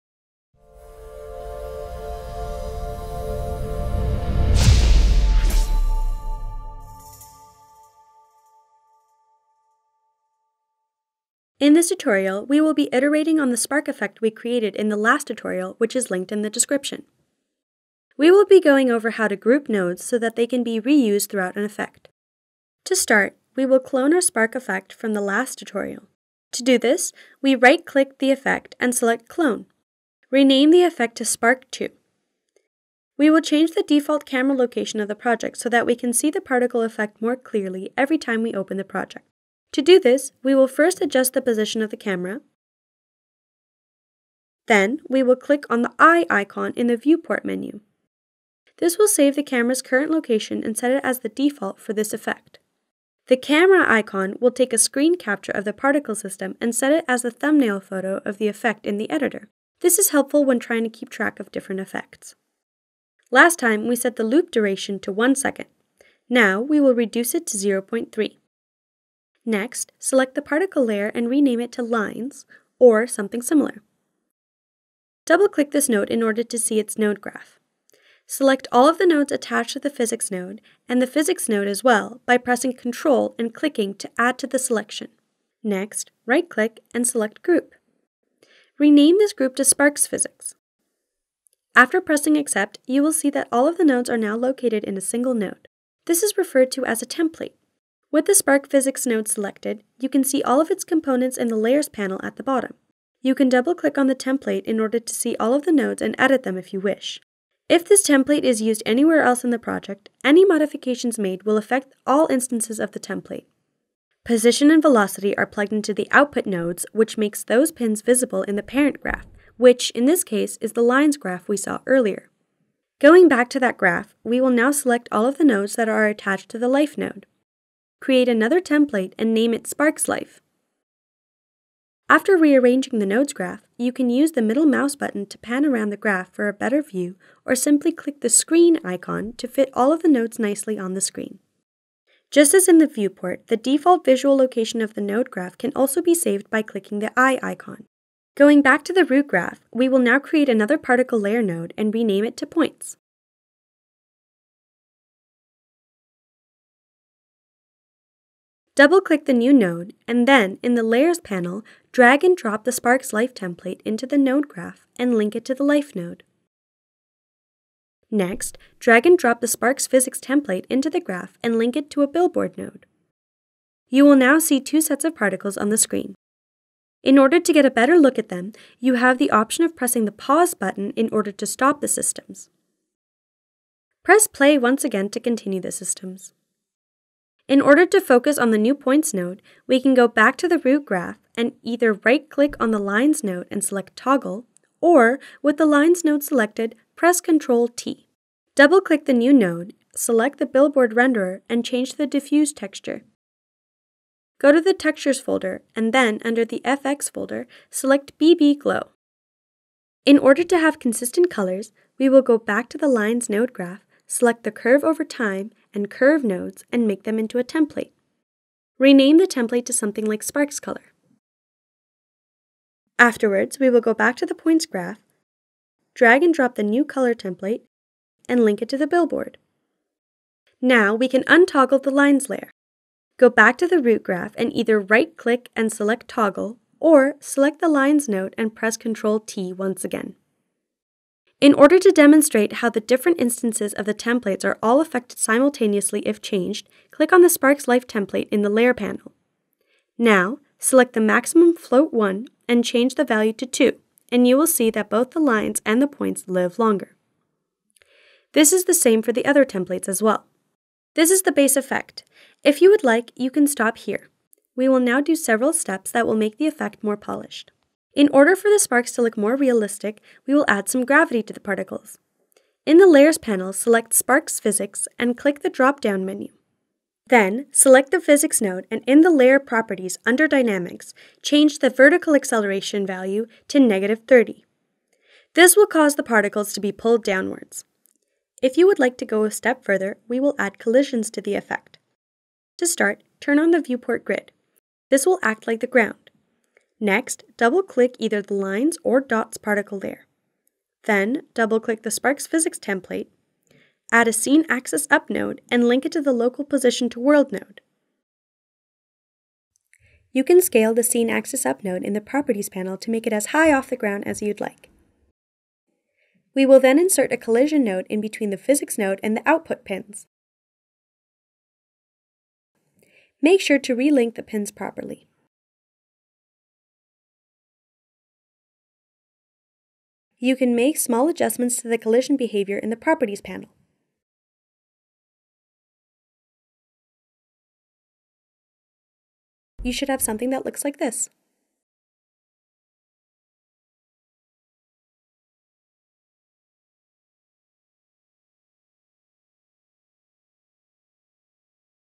PopcornFX Basics - Tutorial #4: Introduction to templates
This fourth tutorial for beginners will teach you everything you need to know to get started with PopcornFX v2.
Let's discover the templates package included in the editor!

00:12.4 0. Check PopcornFX Basics - Tutorial #3
00:27.3 1. Clone a VFX
00:45.0 2. Save the Camera Position
01:06.2 3. Change the loop Delay
01:11.1 4. Set the Layer's Name to "Lines"
01:25.5 5. Group the Positions  & Velocities Nodes
02:28.3 6. Create a New Particle Layer
02:41.6 7. Use the previously Created Template
03:21.4 8. Change the Diffuse Texture
04:12.3 9. Change the Value inside the Template
04:24.5 10. And Now , Let's Make It Pretty
04:32.5 11. Add Acceleration to the Template
05:03.8 12. Change the Initial Position
05:16.2 13. Add Collision to the Physics